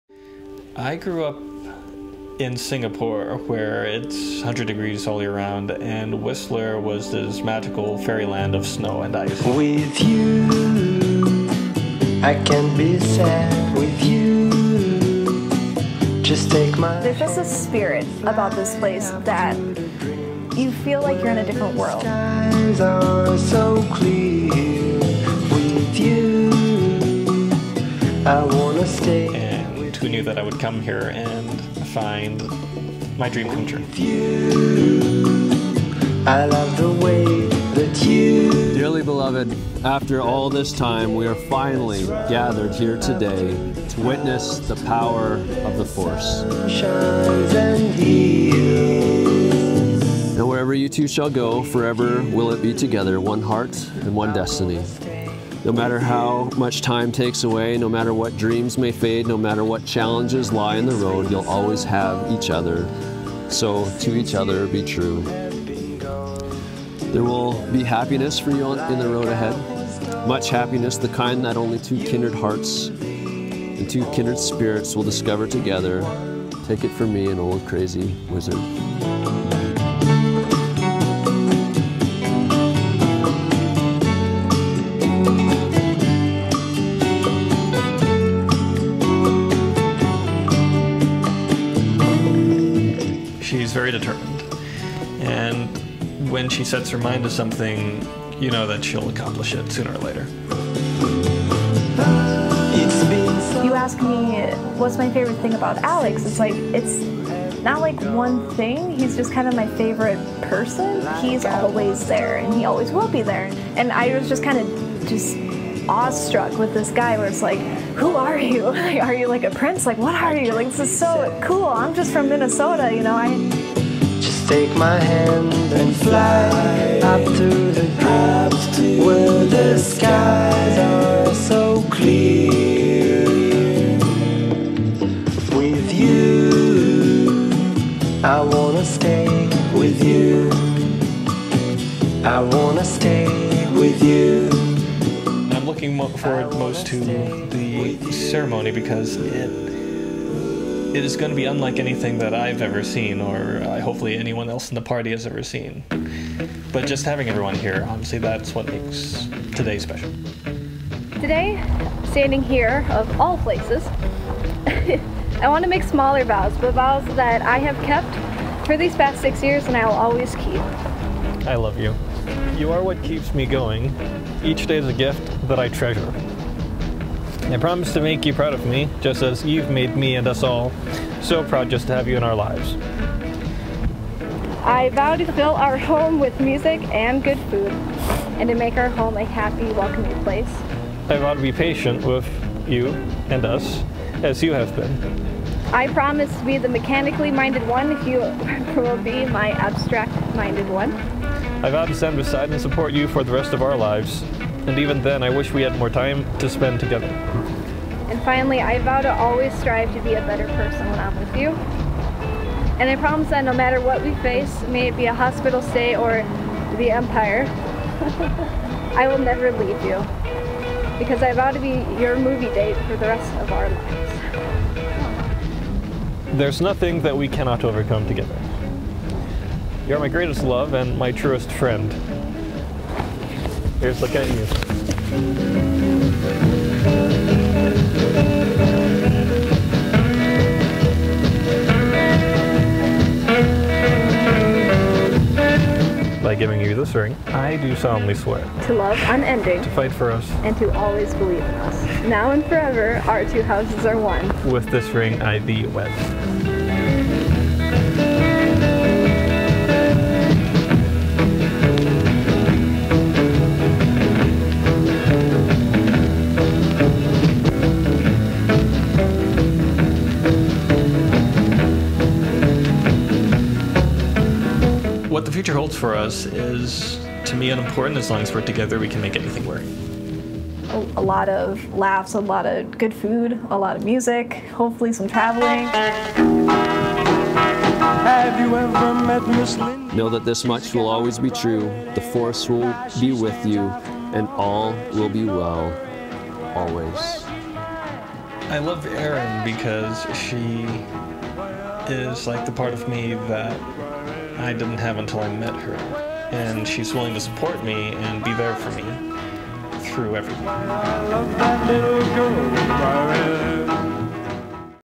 Erin & Alex | Whistler Wedding | Paul Cameron Productions
Erin & Alex tied the knot in an extremely personal and unique way with a destination wedding in their one of their favourite fairy tale locations: Whistler, Canada. Their wedding featured a being wed by a Jedi at the famous Stone Circle in the village, a cocktail reception with all of their favourite movie paraphernalia, a luxurious dinner at the Fairmont, and a private screening of one of their favourite flicks at the Squamish Cultural Centre. What a special day!